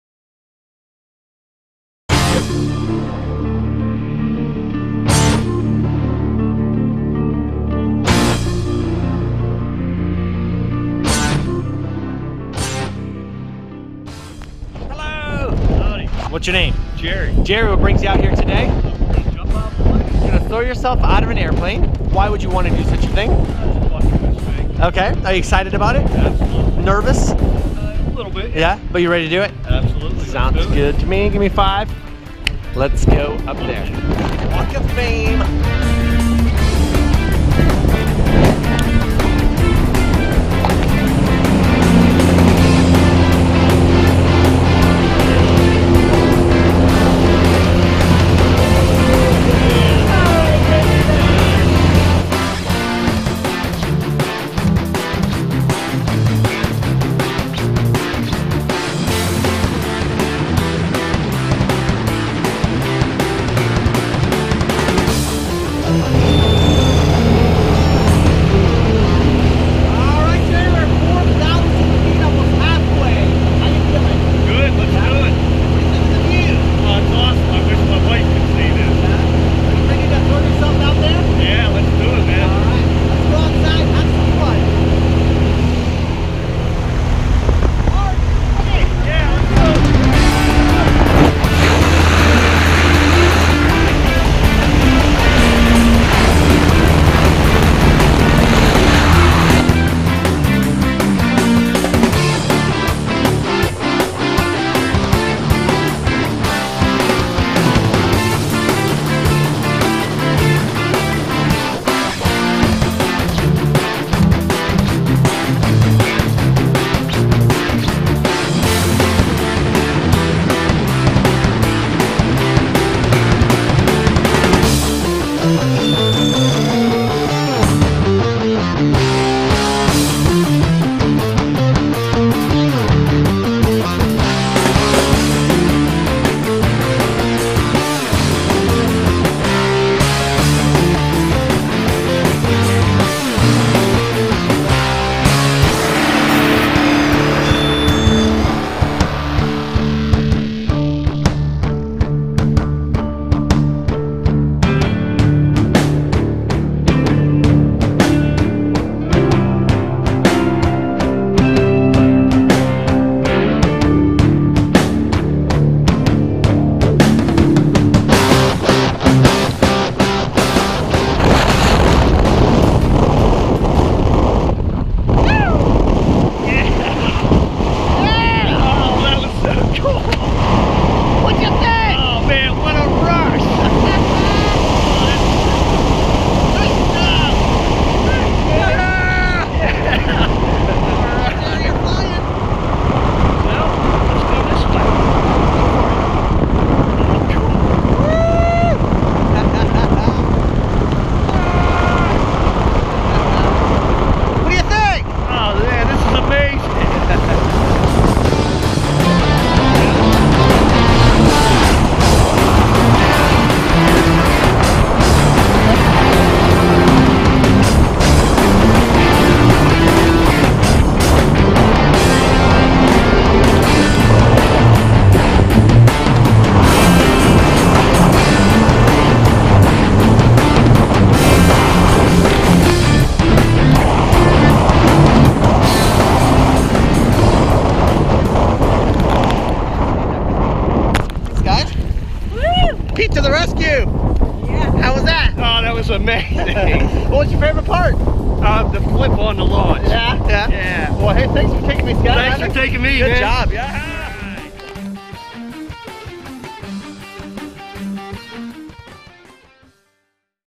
Jerry Moore at Skydive OBX
My first skydive, done at Skydive OBX, while on vacation in the Outer Banks of North Carolina, 9-10-17.